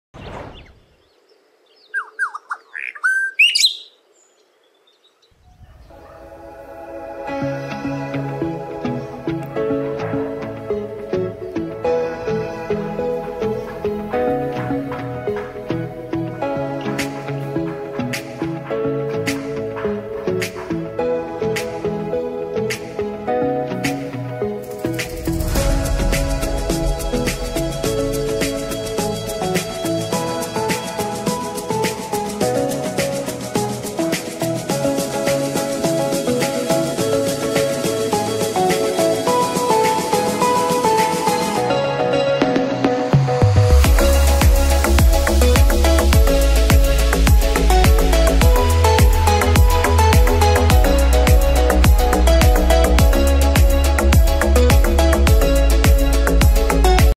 Linda Chácara
Chácara com 5.000,00 m², com 02 açudes, árvores frutíferas, poço artesiano e casa de alvenaria com 120,00 m², de 03 dormitórios, sala de estar, sala de jantar, cozinha, banheiro social, banheiro de serviço, área de serviço, despensa, garagem para 02 carros e churrasqueira.
Tecelar aconchego ao teu alcance.